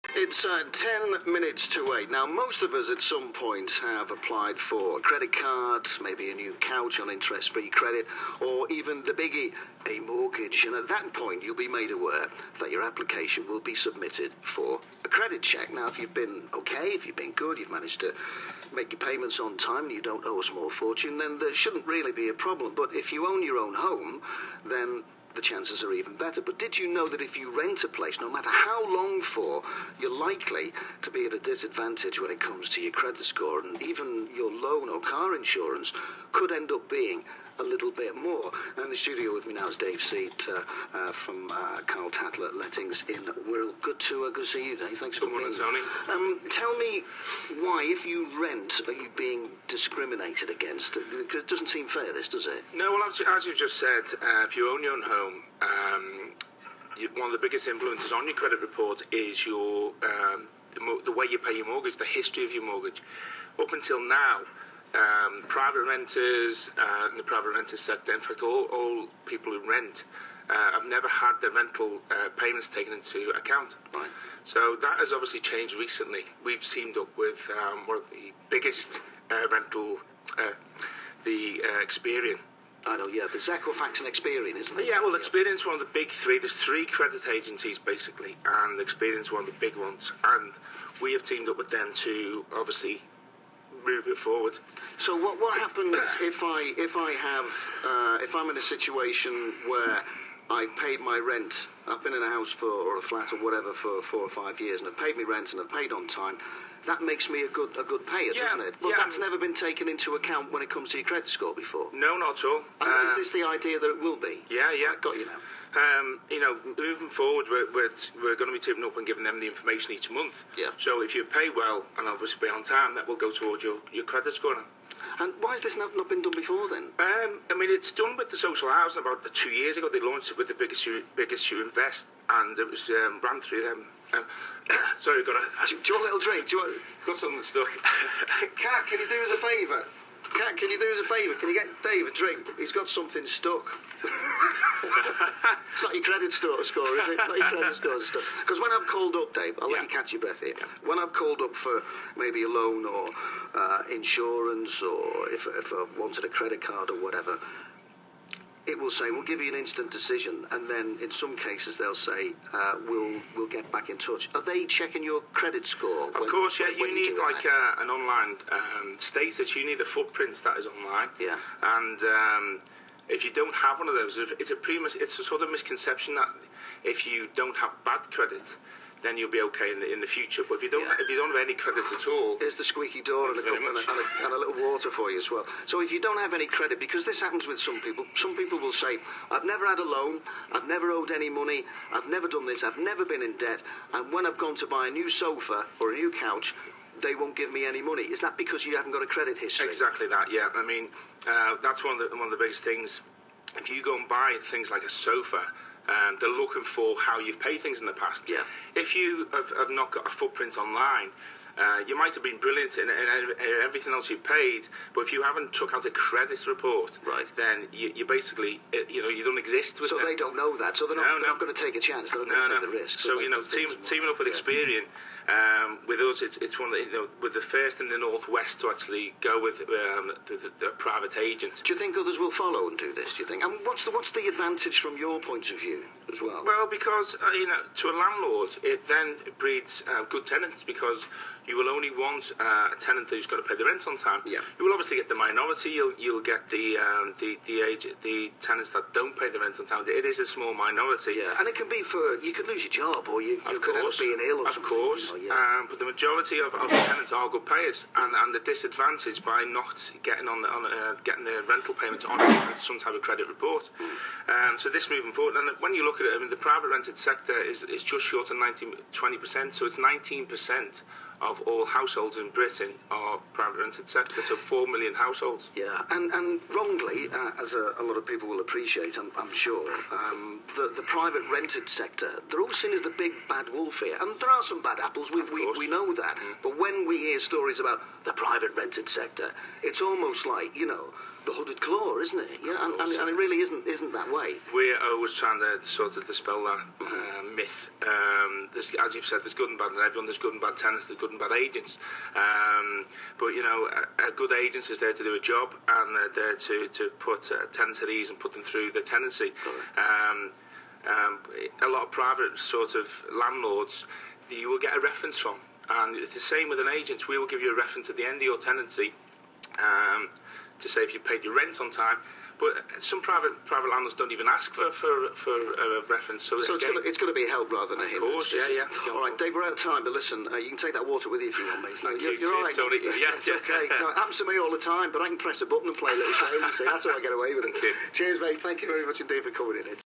Dave Seed on BBC Radio Merseyside discussing the Rental Exchange
Did you know that paying your rent is not recognised for building up your credit rating? Well with the Experian Rental you build up your credit rating just by paying you rent on time...and Karl Tatler Lettings are the only lettings agent in the North West to join the Rental Exchange.